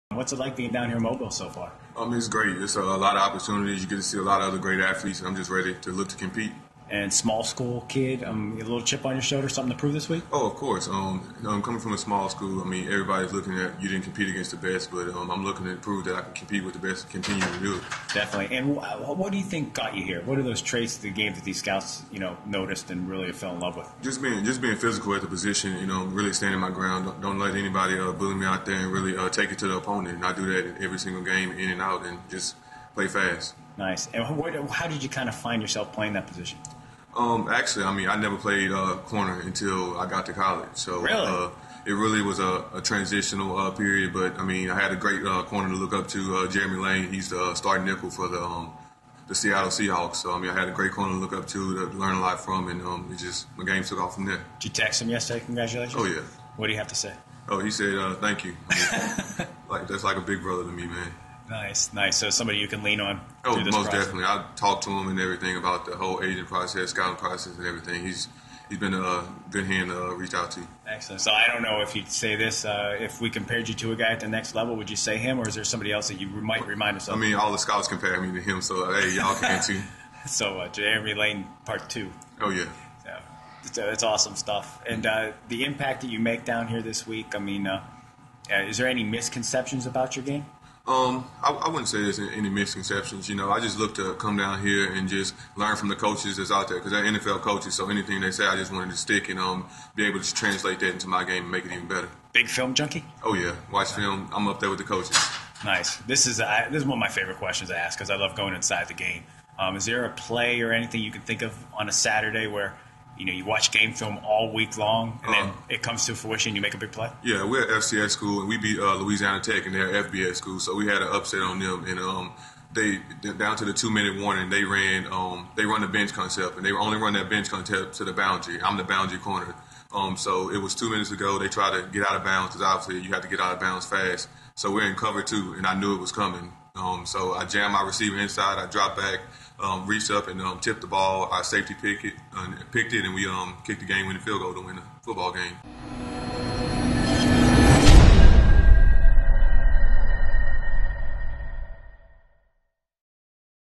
Imoan Claiborne – Northwestern State – 2015 Reese’s Senior Bowl
Northwestern State cornerback Imoan Claiborne talks with Football Insiders about being named to the 2015 Reese’s Senior Bowl, what he hopes to showcase this week.